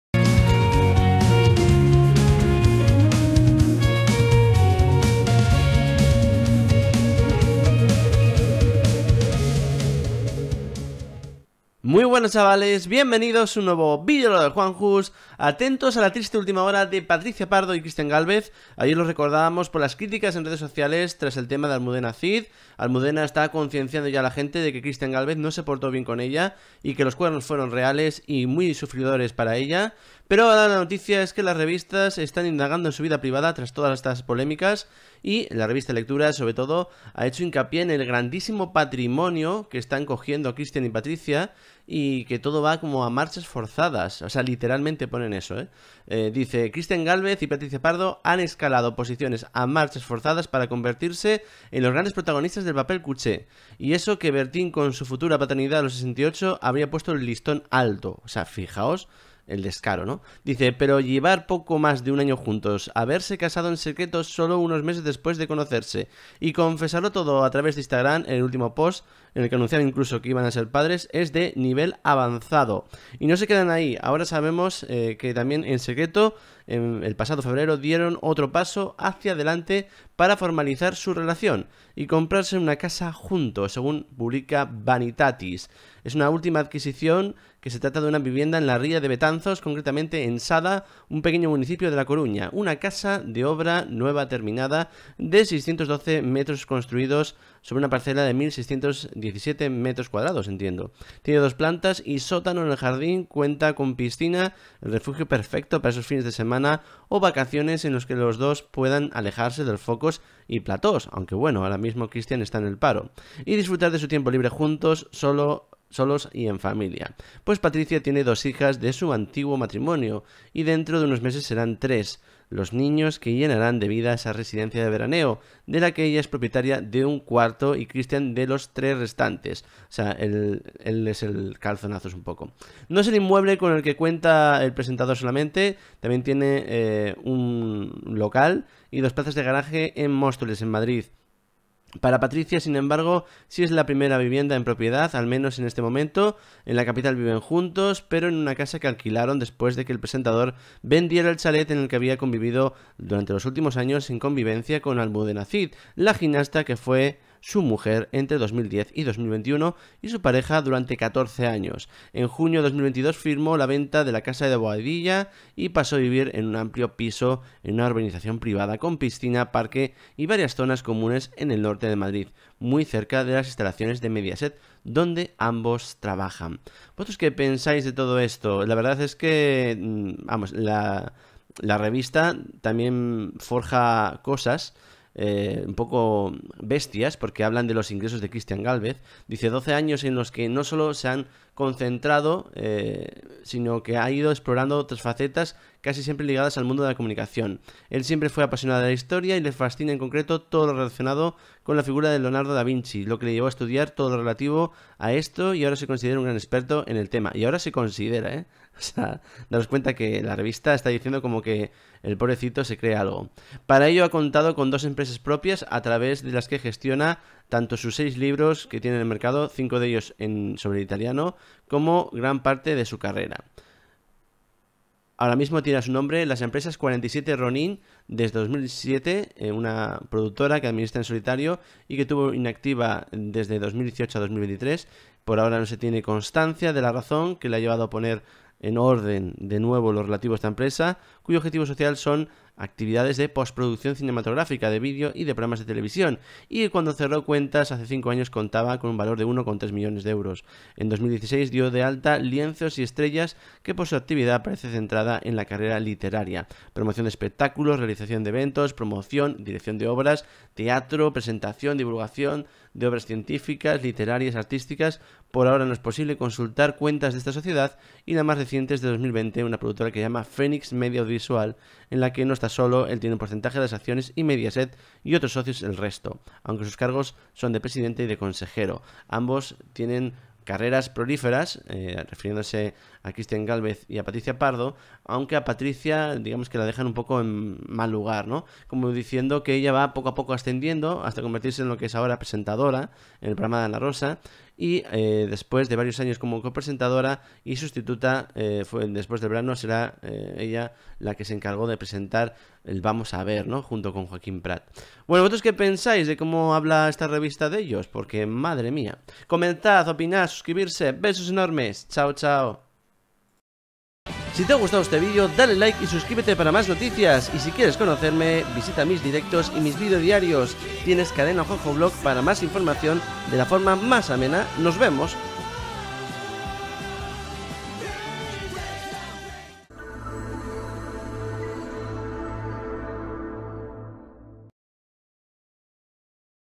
Triste última hora para Christian Gálvez y Patricia Pardo
El popular presentador de telecinco y mediaset anda pasando por una etapa muy complicada que os contamos ahora .
El programa 25 palabras ha sido un fracaso y la cancelación de Pasapalabra le ha marcado .